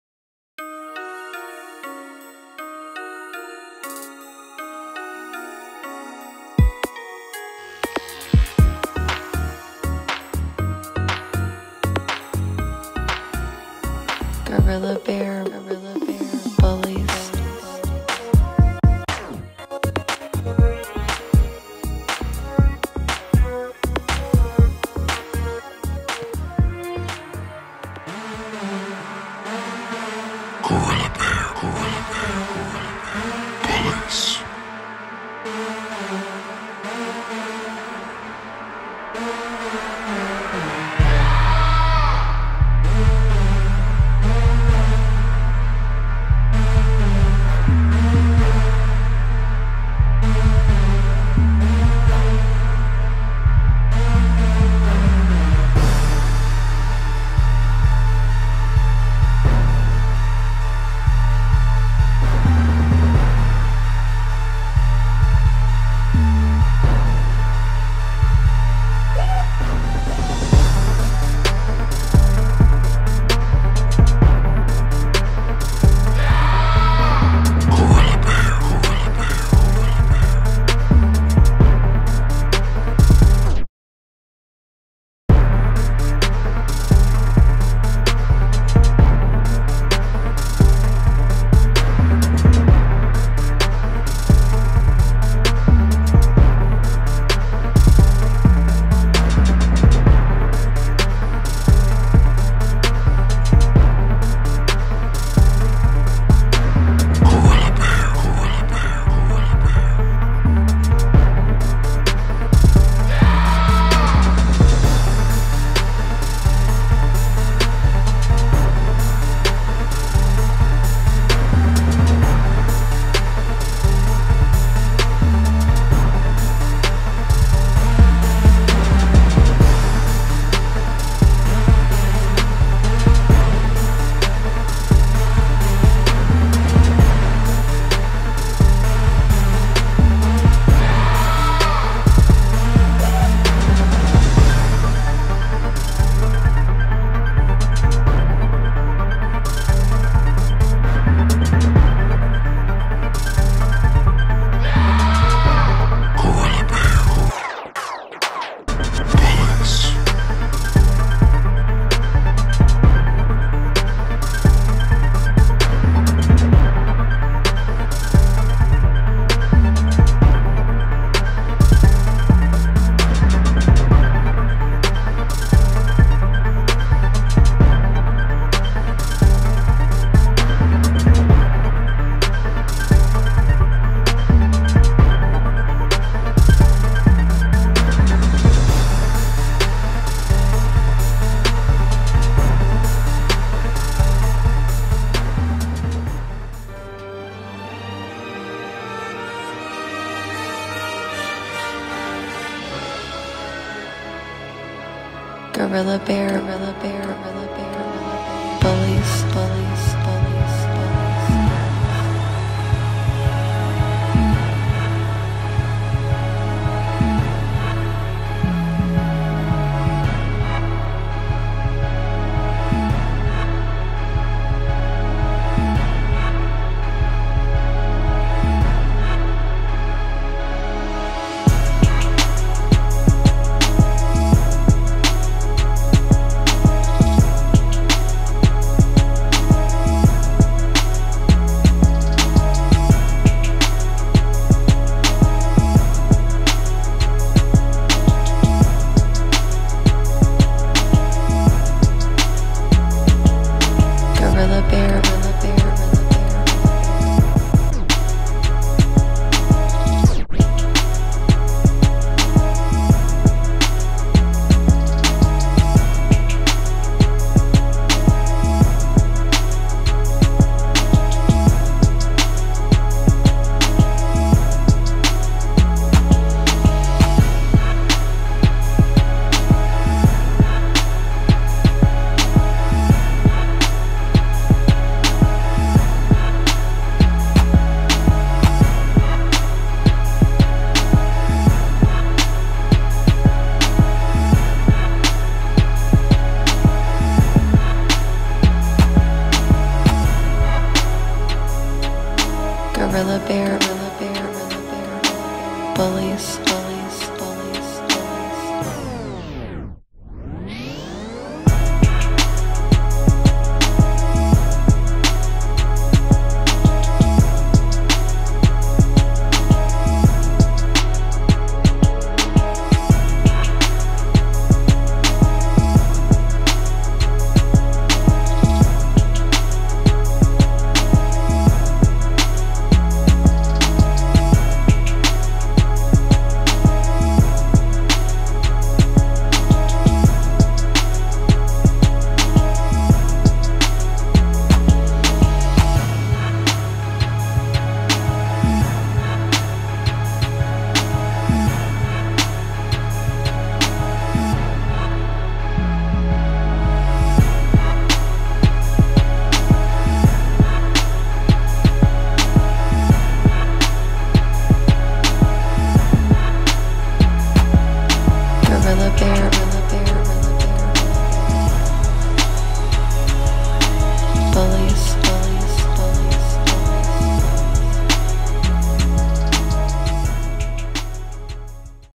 **THESE ARE NOT PIT BULLS** [must watch] (Gorilla Bear Bullies The Yard)
On set checking out some of Sam Winston’s yard over at Gorilla Bear Bullies (GBB). Featured are some fine specimens of the American Bully breed which are owned and produced right over at Gorilla Bear Bullies located in Virginia.